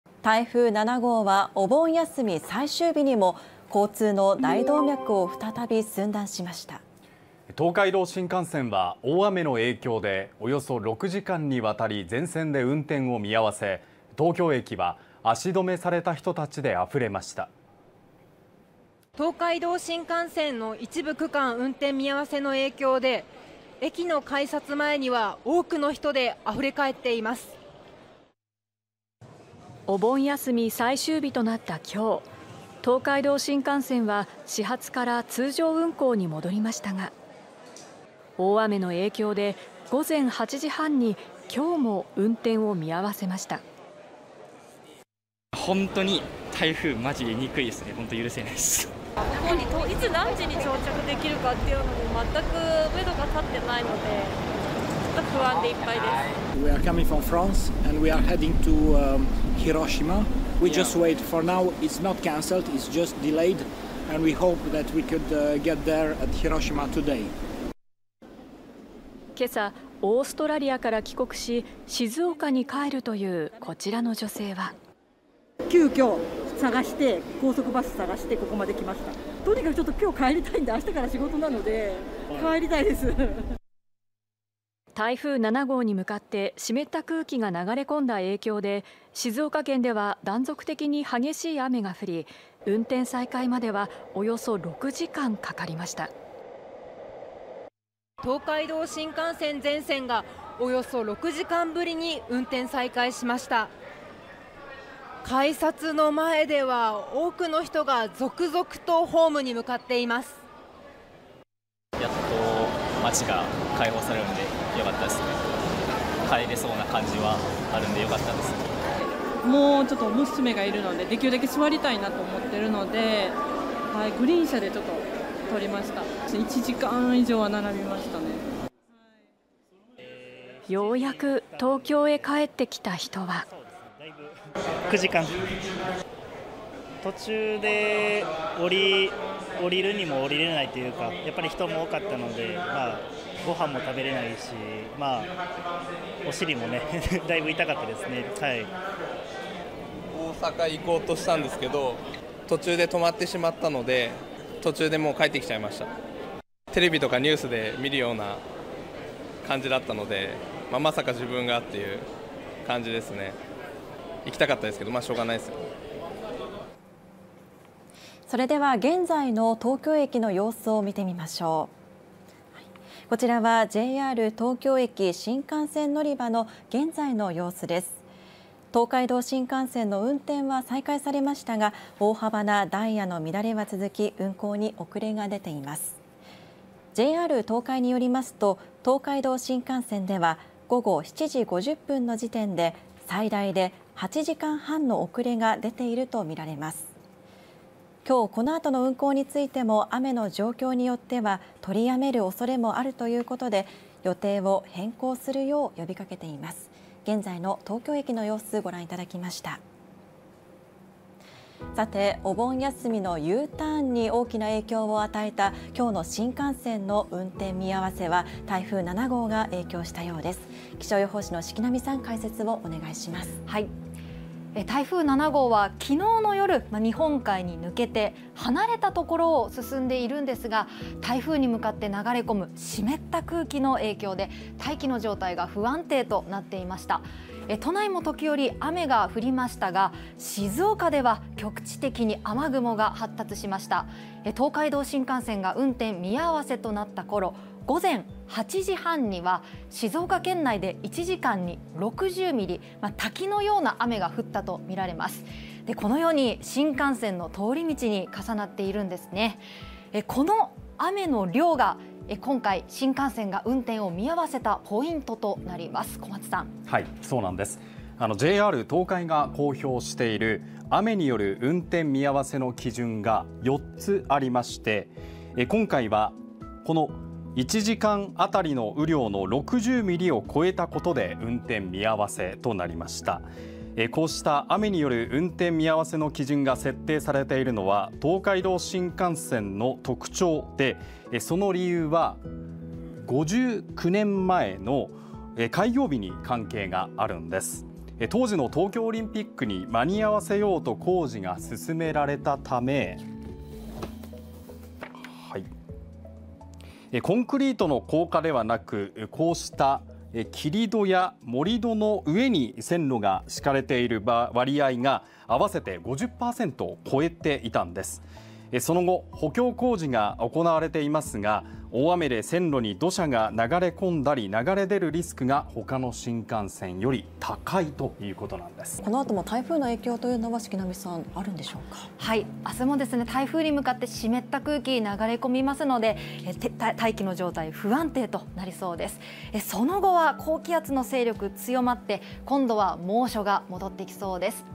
東海道新幹線 一時全線で運転見合わせ 東京駅は大混雑／その理由を解説
台風7号はお盆休み最終日にも交通の大動脈を再び寸断しました。東海道新幹線は、大雨の影響で、約6時間に渡り全線で運転を見合わせ、東京駅は足止めされた人たちで溢れました。
「東海道新幹線の一部区間、運転見合わせの影響で、駅の改札前には、多くの人で溢れかえっています」
お盆休み最終日となったきょう（16日）、東海道新幹線は始発から通常運行に戻りましたが…大雨の影響で午前8時半にきょう（16日）も運転を見合わせました。
「本当に台風まじで憎いです」「大阪にいつ何時に到着できるか全く目処が立っていないので、不安でいっぱいです。」「フランスから来ました。広島に行きます」「私たちはただ待つだけキャンセルになっていないので私たちは今日中に行けることを祈ります。」
今朝オーストラリアから帰国し、静岡に帰るというこちらの女性は…。「急きょ高速バス探してここまで来ました。」「とにかくちょっときょう帰りたいので、あしたから仕事なので帰りたいです」
台風7号に向かって、湿った空気が流れ込んだ影響で、静岡県では断続的に激しい雨が降り、運転再開までは、約6時間かかりました…。
「東海道新幹線が全線が約6時間ぶりに運転再開しました。改札の前では多くの人が続々とホームに向かっています」
「やっと待ちが解放されるので帰れそうな感じはあるのでよかったです」「娘がいるのでできるだけ座りたいと思ってるのでグリーン車を取りました。1時間以上は並びましたね」
ようやく東京へ帰ってきた人は…「9時間くらいです。途中で降りるに降りられないお尻がだいぶ痛かった」」「大阪に行こうとしたが途中で止まったので帰ってきちゃいました」「テレビで見るような感じだったのでまさか自分がという感じですね」「行きたかったけど仕方ない」
それでは現在（16日午後8時過ぎ）の東京駅の様子を見てみましょう。こちらはJR東京駅・新幹線乗り場の現在の様子です。東海道新幹線の運転は再開されましたが、大幅なダイヤの乱れは続き運行に遅れが出ています。JR東海によりますと東海道新幹線では午後7時50分の時点で最大で8時間半の遅れが出ているとみられます。きょう（16日午後8時以降）この後の運行についても、雨の状況によっては取りやめる恐れもあるということで、予定を変更するよう呼びかけています。
お盆休みのＵターンに大きな影響を与えたきょう（16日）の新幹線の運転見合わせは、台風7号が影響したようです。気象予報士の敷波さん、解説をお願いします。
台風7号はきのう夜（15日夜）、日本海に抜けて離れたところを進んでいるんですが、台風に向かって流れ込む湿った空気の影響で大気の状態が不安定になっていました。
都内も時折、雨が降りましたが、静岡では局地的に雨雲が発達しました。東海道新幹線が運転見合わせとなったころ…
午前8時半には静岡県内で1時間に60ミリ…滝のような雨が降ったところがありました。このように…新幹線の通り道に重なっています。この雨の量が今回、新幹線が運転を見合わせたポイントになります。
JR東海が公表している雨による運転見合わせの基準が4つありまして、今回はこの1時間当たり雨量の60ミリを超えたことで運転見合わせとなりました。
こうした雨による運転見合わせの基準が設定されているのは東海道新幹線の特徴で、その理由は…59年前の開業日に関係があるんです。当時の東京オリンピックに間に合わせようと工事が進められたため、コンクリートの高架ではなく、こうした切り土や盛り土の上に線路が敷かれている割合が50%を超えていたんです。その後、補強工事が行われていますが、大雨で線路に土砂が流れ込んだり、流れ出るリスクが他の新幹線より高いんです。
敷波さん、この後も台風の影響はまだあるんでしょうかか？
あす（17日）も、日本海の台風に向かって南から湿った空気が流れ込むので急にざっと雨が強まったり、雷雨になったりすることがありそうです。その後は、高気圧が勢力を強めて猛烈な暑さが戻ってきそうです。